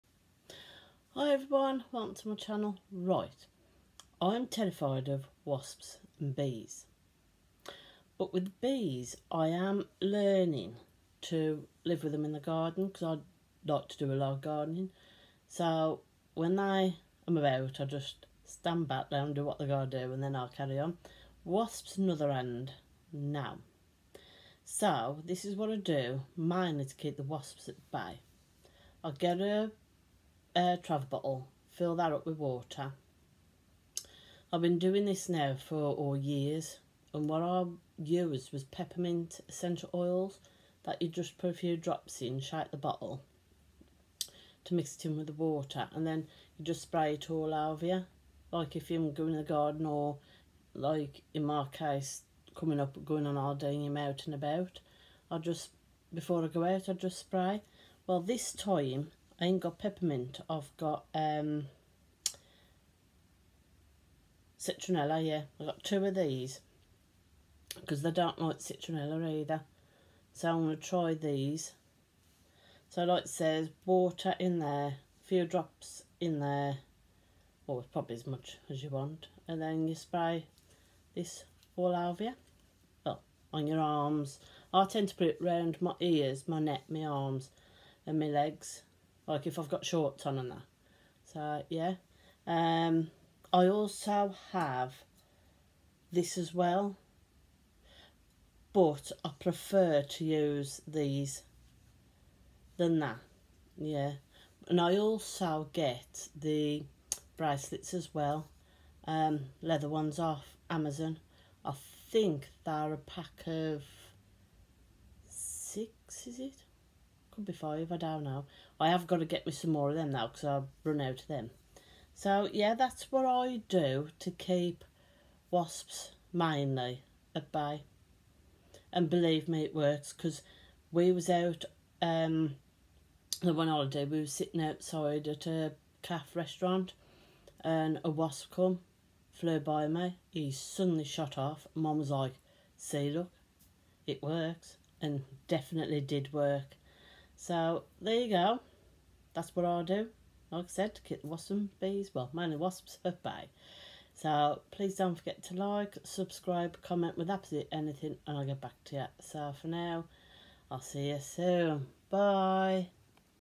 How I cope with wasps and bees
Hi everyone. I’m terrified of wasps and bees. This is how I cope with them.